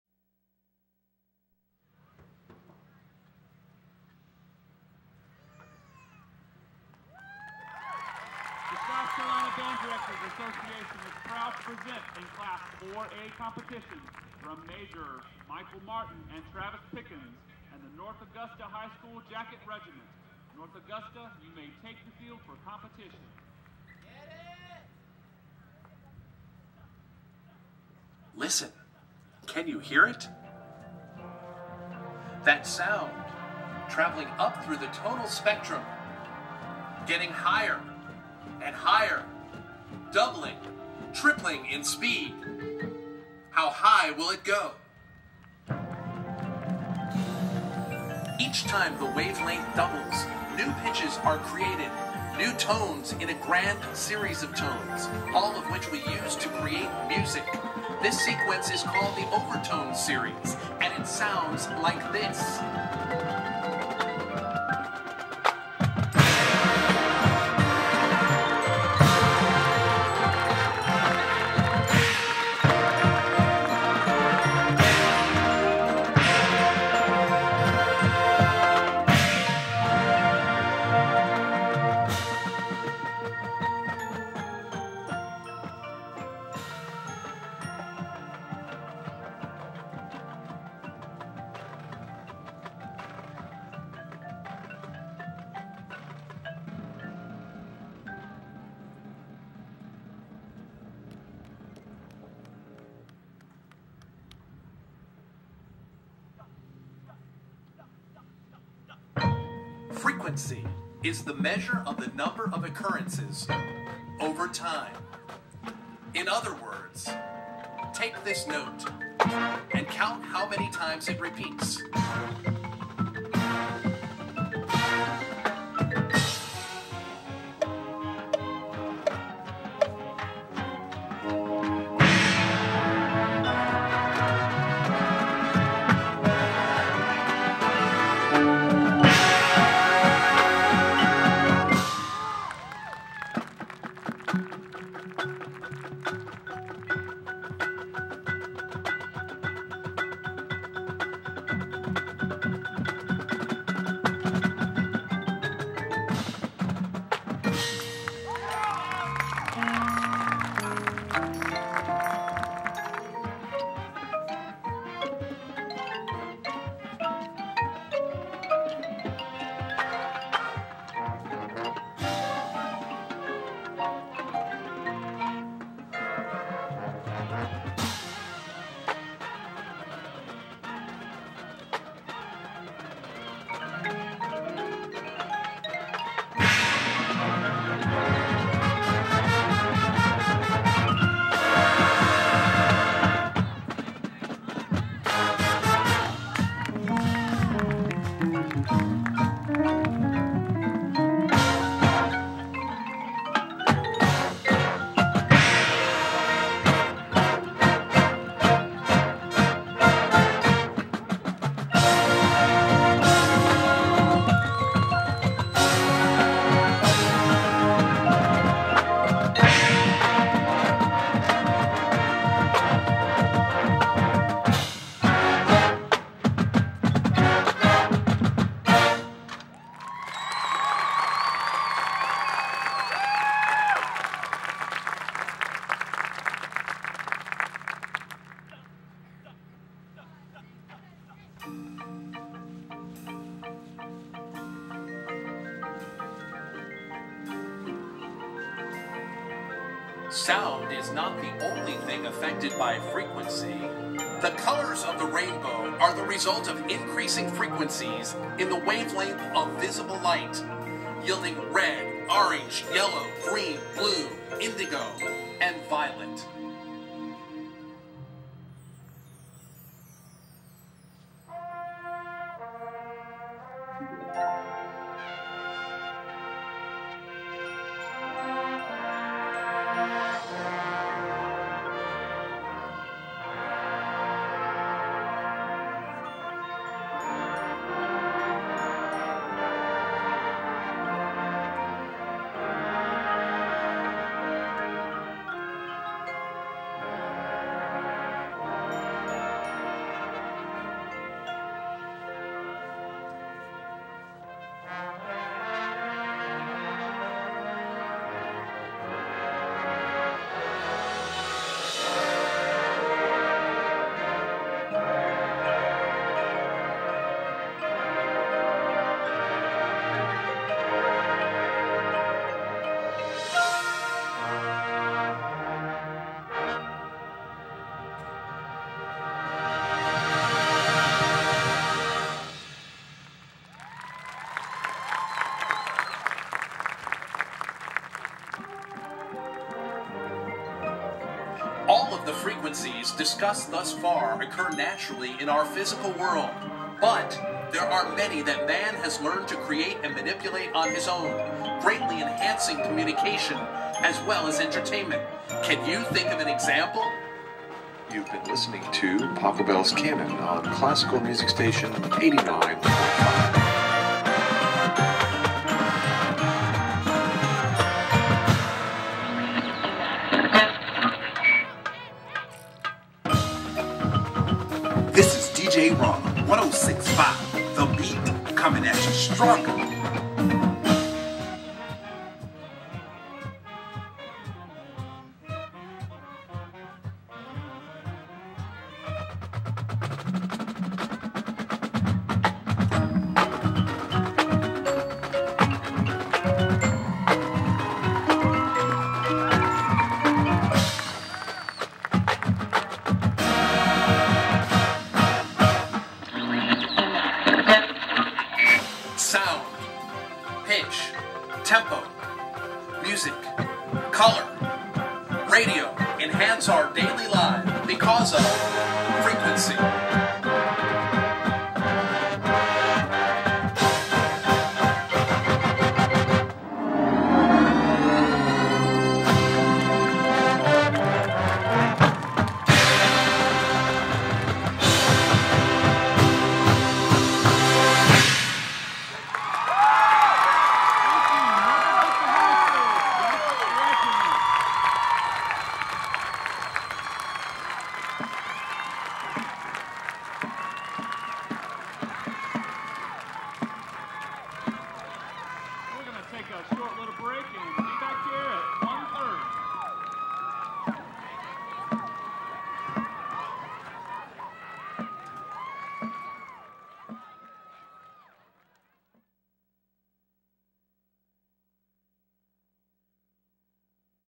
Jacket Regiment - Lower State Band Championships
2010 Jacket Regiment - 2nd Place 4A
Lower State Band Championships
West Ashley High School
Charleston, SC